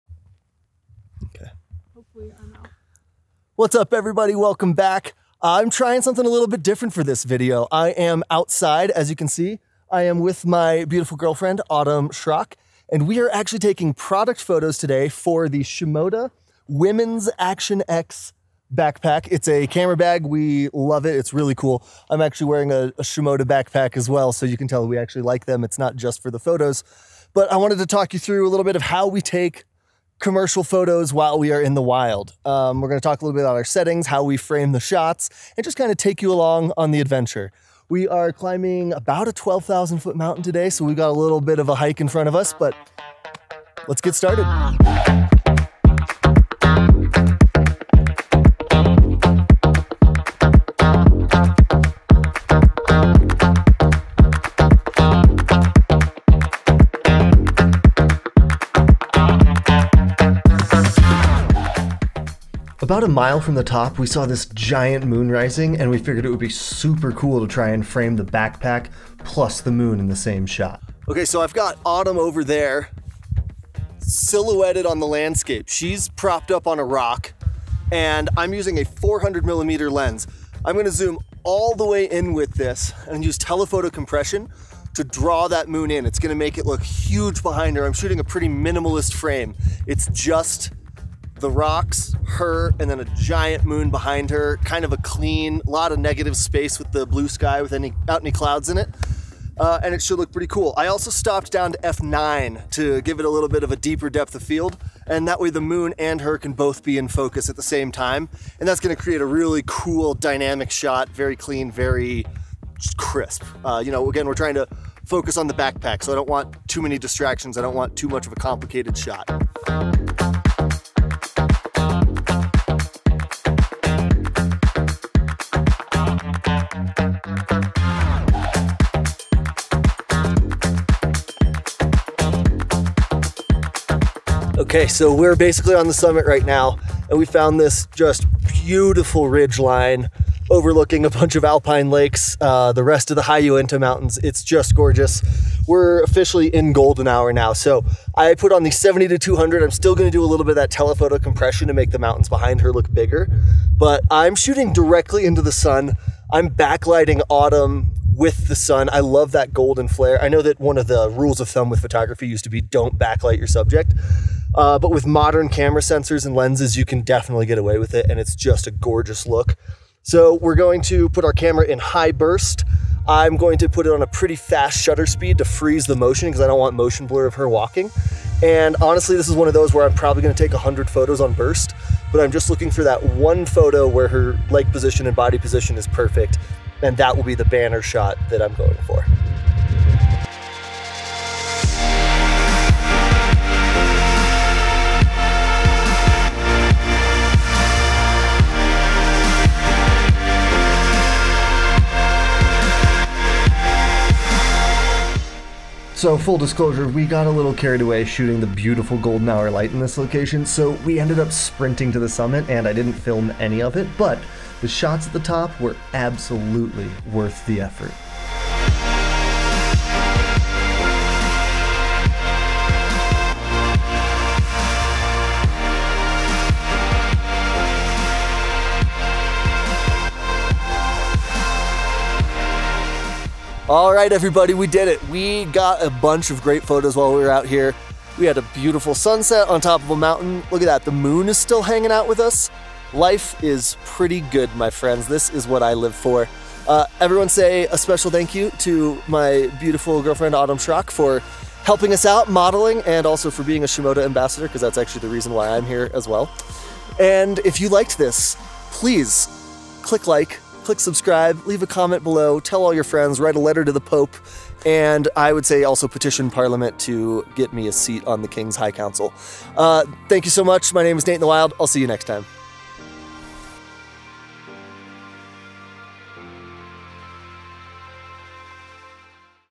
Behind The Scenes of a Commercial Nature Photoshoot
The ALL NEW Women's Action-X bag is here, and it is a BANGER. I was tasked with creating some of the imagery used for the launch, so I headed into the mountains to show off this bag in the high alpine where it belongs. In this video I'll explain what I'm doing and why as I create the banner image for the launch of this new photography product.

Used to Film This Episiode:
Cameras:
Sony A1
Sony a7S III

Lenses:
Sony 14mm f/1.8 GM
Sony 24-70 f/2.8 GM II
Sony 20mm f/1.8 GM